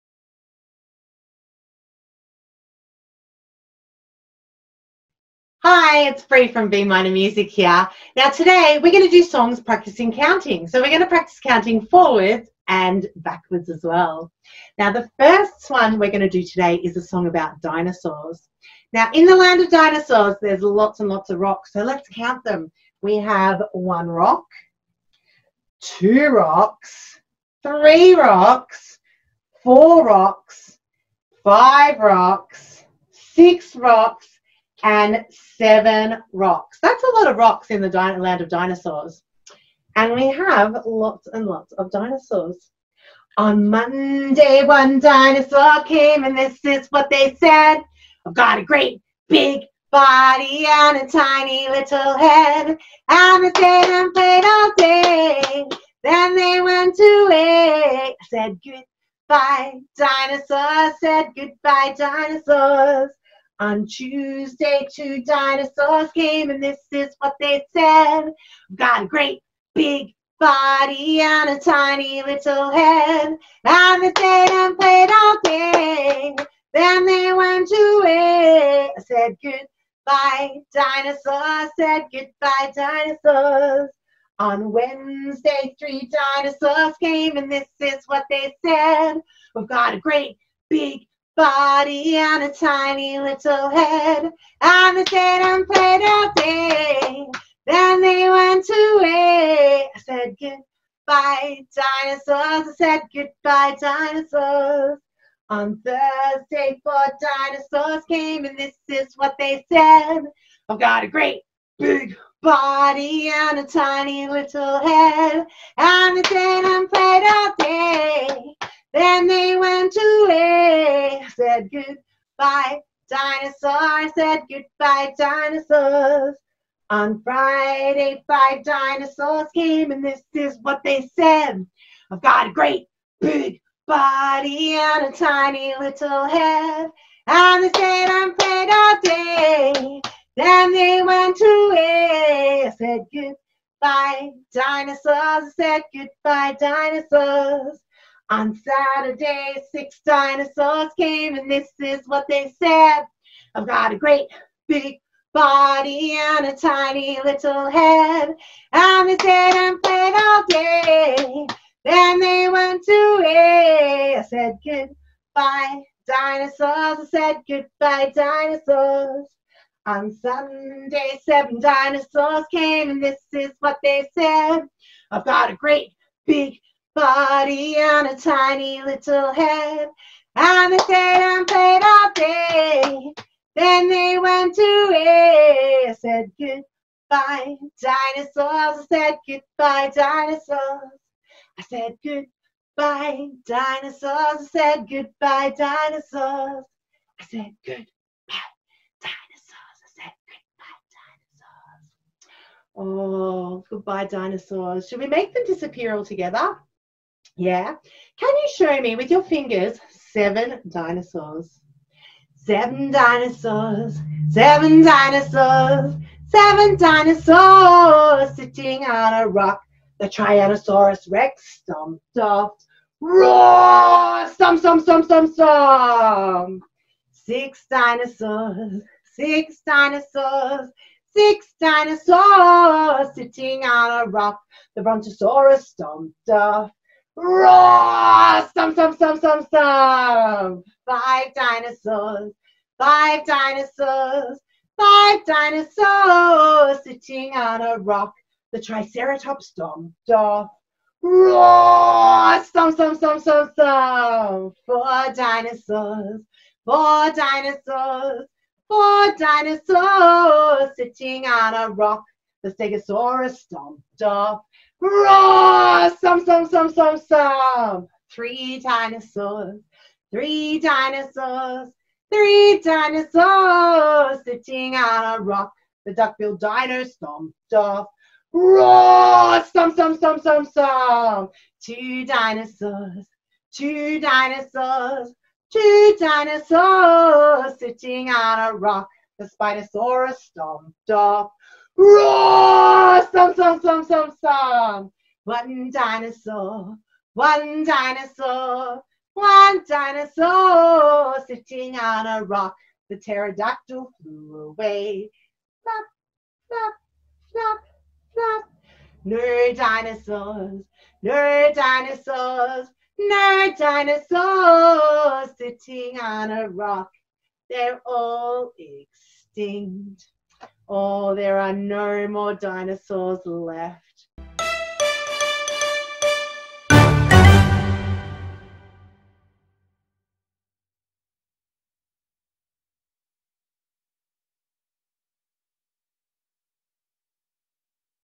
Counting with Dinosaurs
Sing, dance and count along with Bree from B Minor Music whilst she explores counting, the days of the week and dinosaurs whilst practicing motor skills through dancing and actions.
By B Minor Music